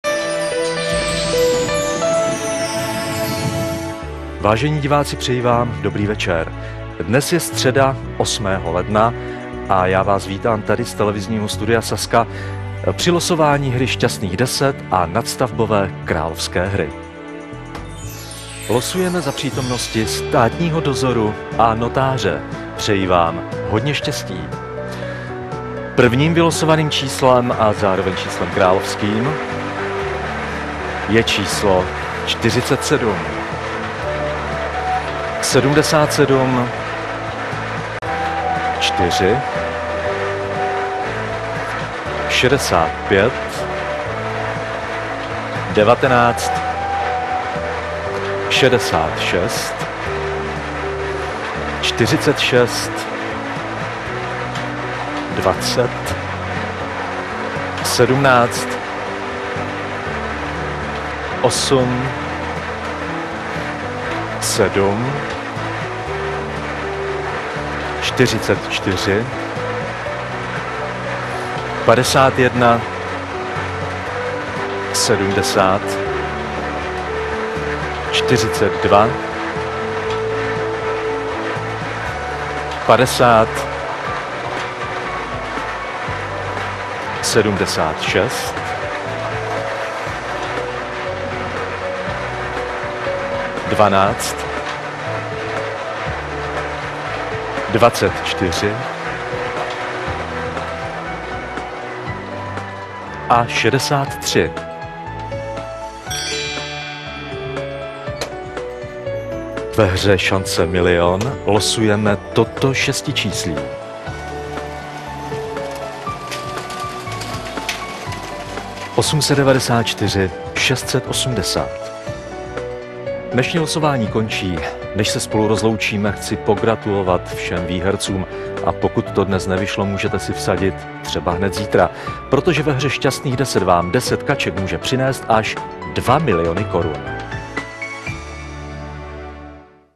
Š10 - 8.1.2014
Záznam losování Šťastných 10 dne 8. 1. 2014